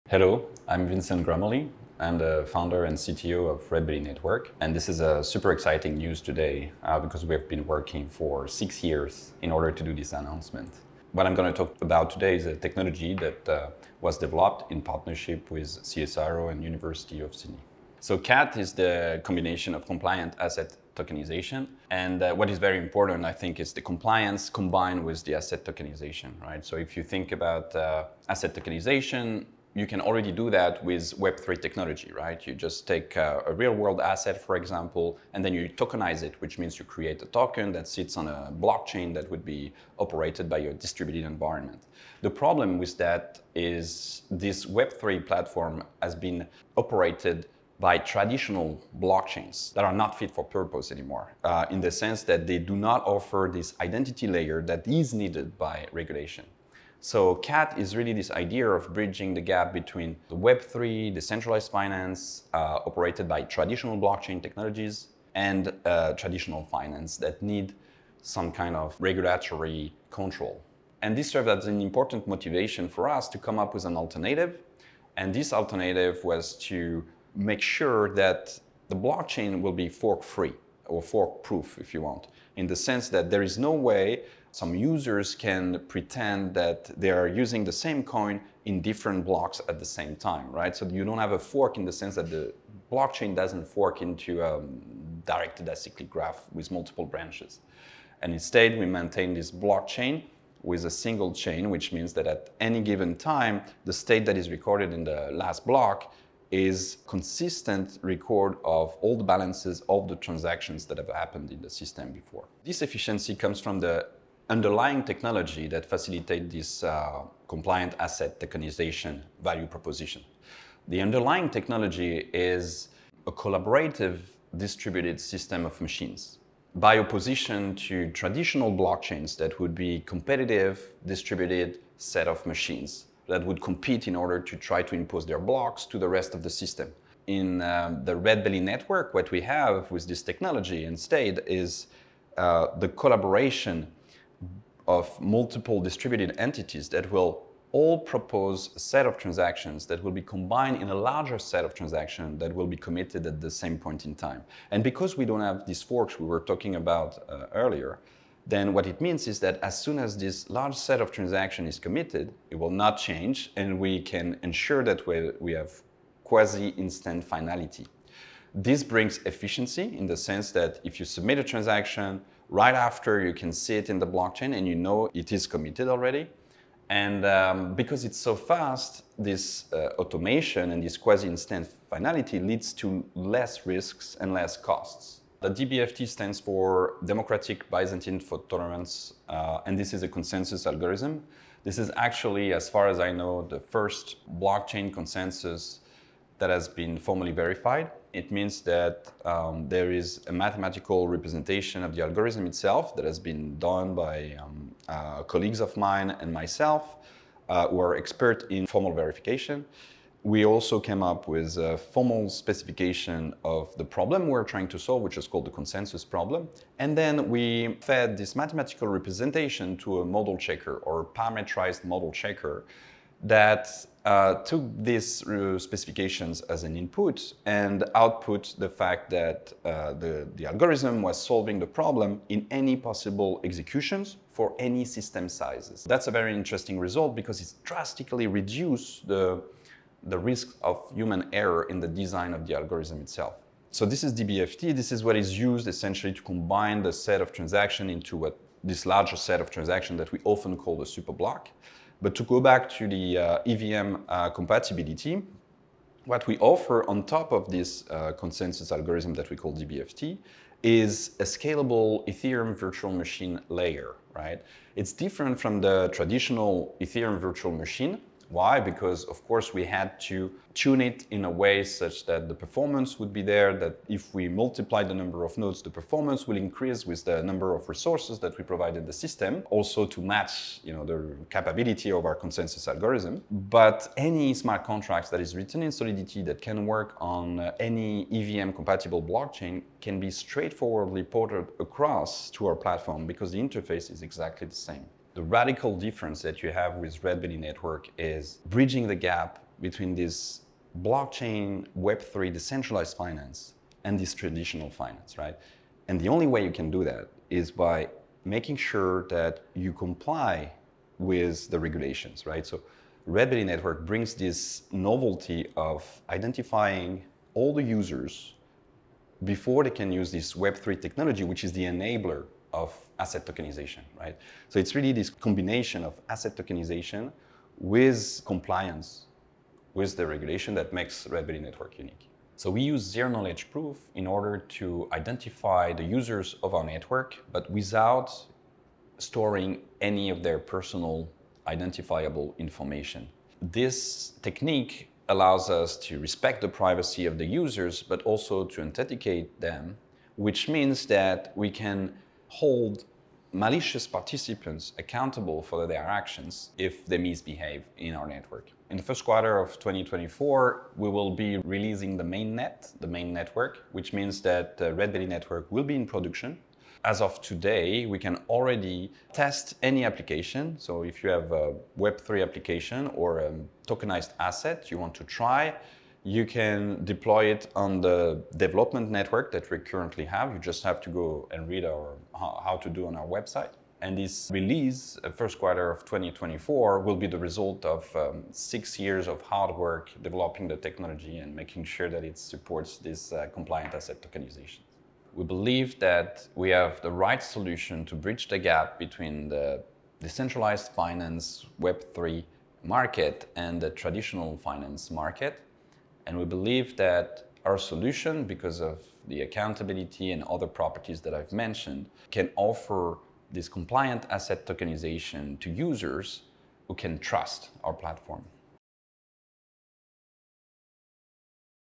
CAT: Redbelly Network Announces Compliant Asset Tokenisation Solution
Redbelly Network's Compliant Asset Tokenisation solution has solved a trillion dollar problem.

“We built Redbelly to contribute to a world where everyone, everywhere has equal access to the economic infrastructure they need.” - Vincent Gramoli, Founder and CTO of Redbelly Network

Using world first technology, Compliant Asset Tokenisation improves access, liquidity, choice and efficiency. By focusing on the dual foundations of composability and accountability, Redbelly puts trustworthy technology at the heart of its Compliant Asset Tokenisation solution.

Hear from Vincent Gramoli (Founder, CTO) and Warren Billington (CEO) about our major announcement today; we're changing the landscape of blockchain technology with Compliant Asset Tokenisation.